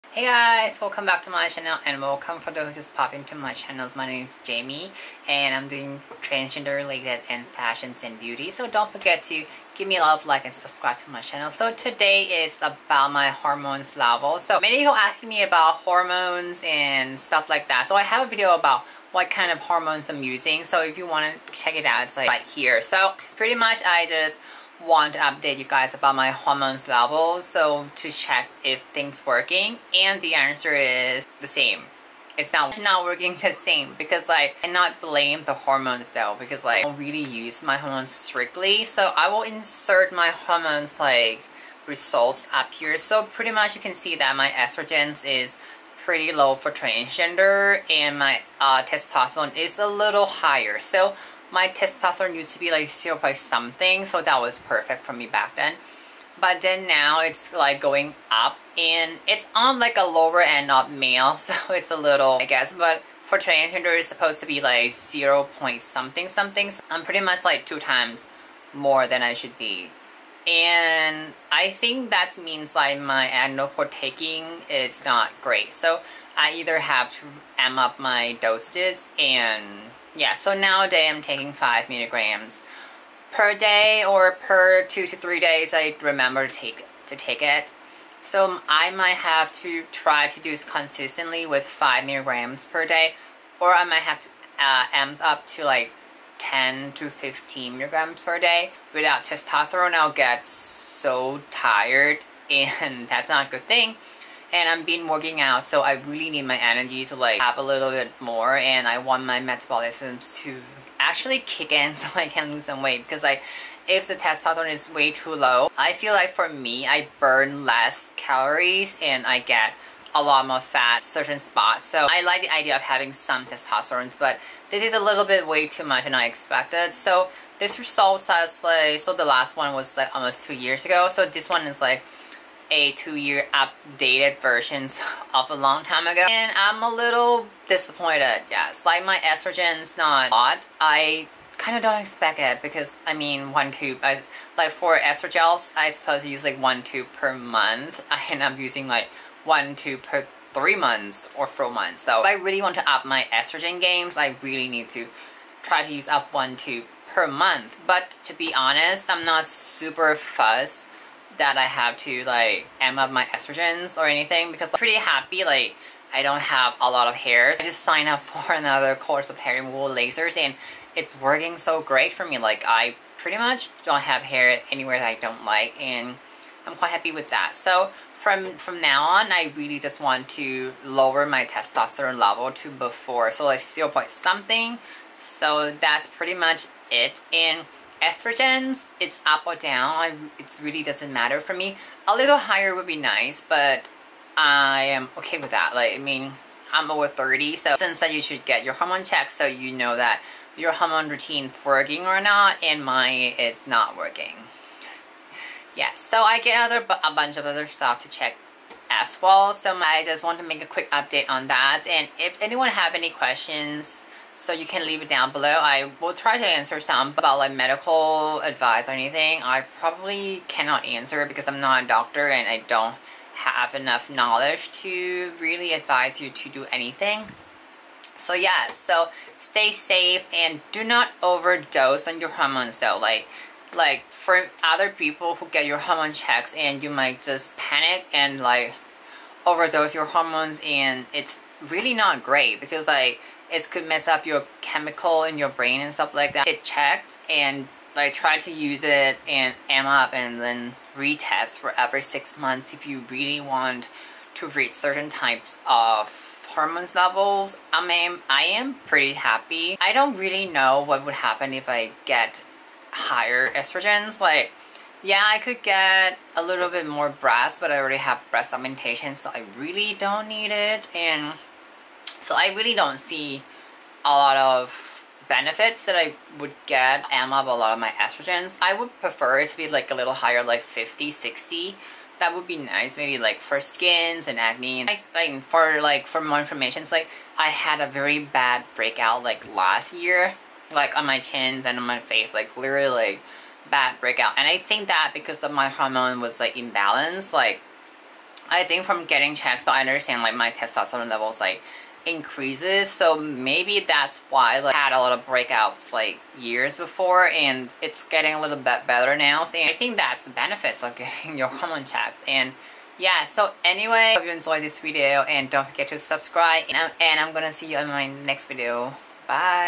My Hormones level MTF HRT 2022
Sooo I’m sharing my hormones level with everyone. But please do use mine as a reference for your situation.
Please consult with your doctor

Instagram jamiiejame